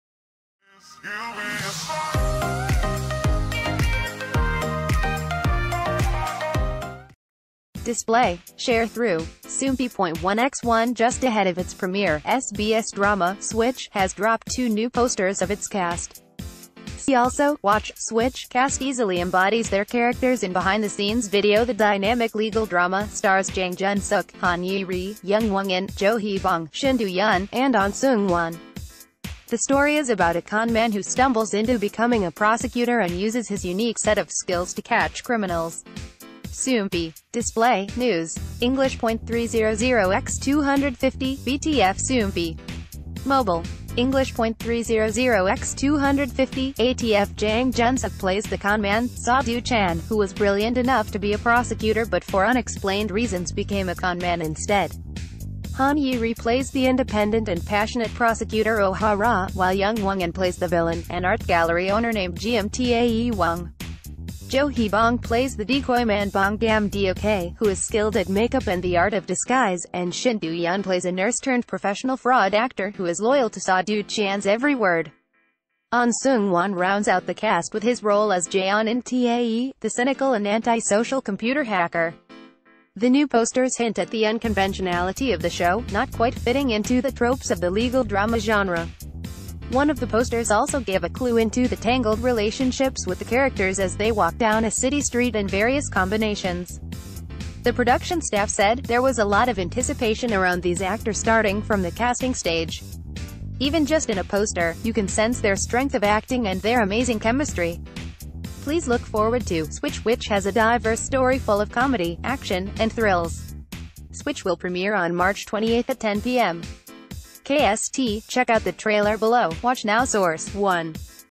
Upcoming Drama "Switch" Starring Jang Geun Suk And Han Ye Ri Reveals Dynamic New Posters
Display. Sharethrough. Soompi.1x1 Just ahead of its premiere, SBS drama “Switch” has dropped two new posters of its cast. See Also: Watch: "Switch" Ca...